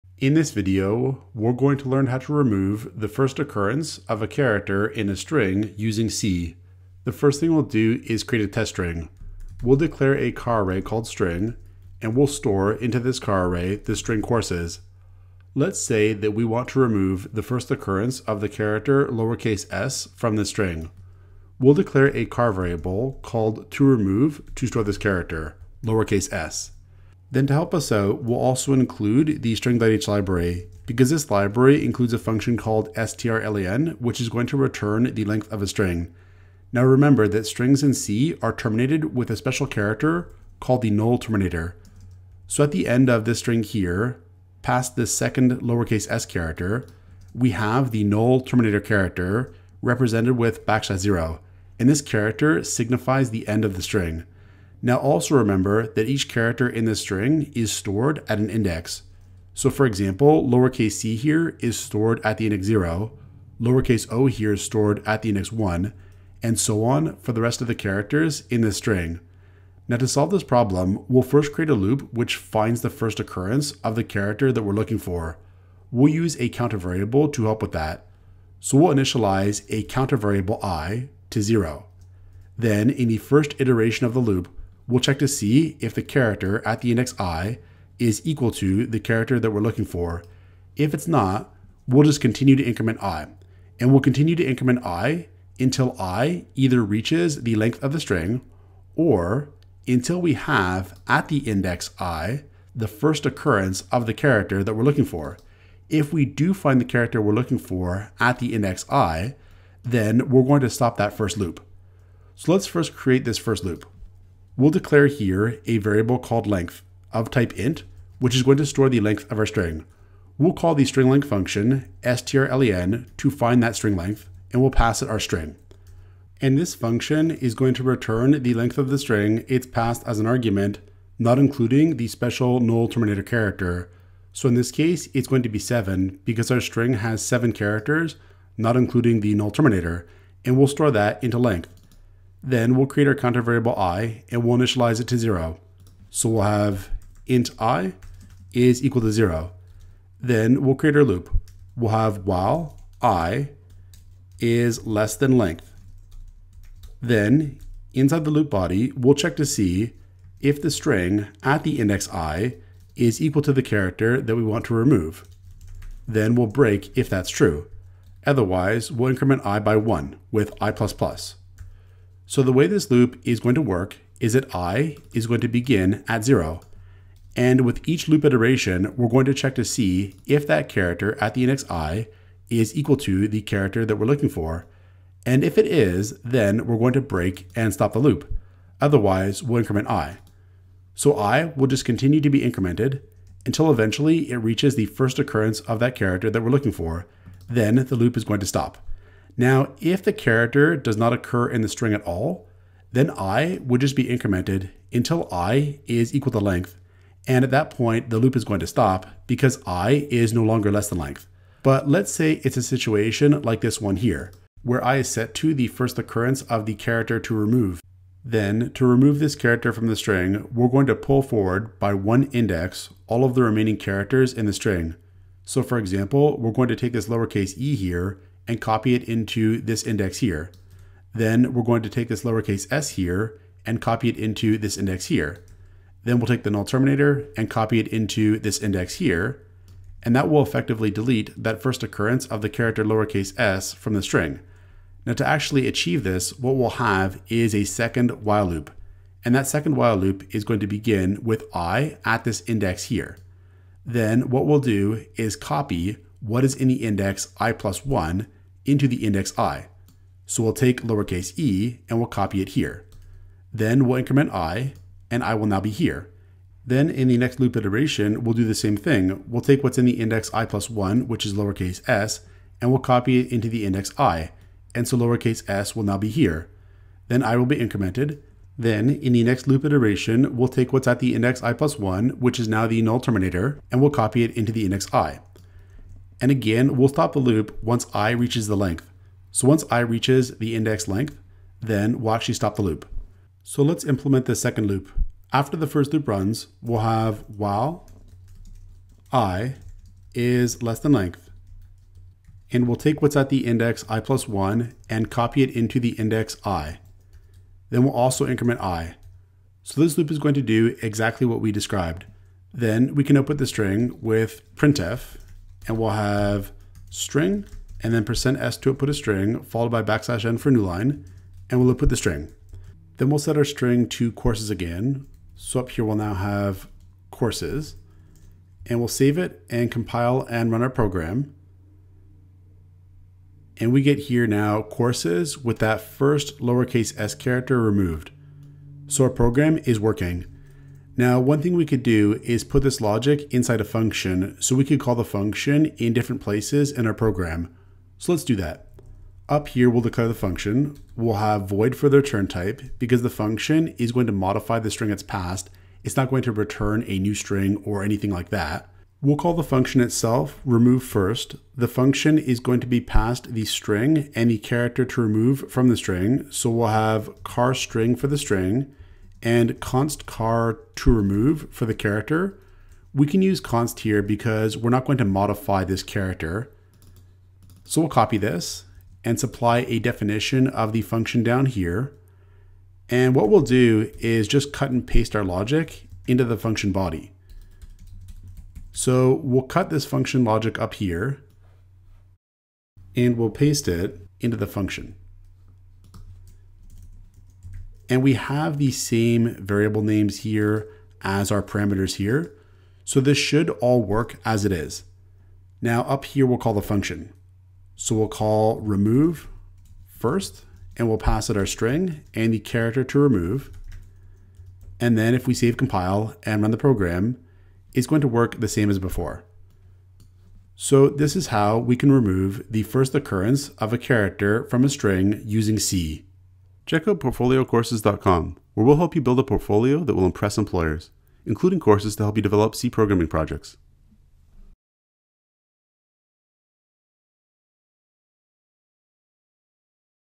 Remove First Occurrence Of A Character From A String | C Programming Example
How to remove the first occurrence of a character from a string using C. Source code to build a portfolio that will impress employers!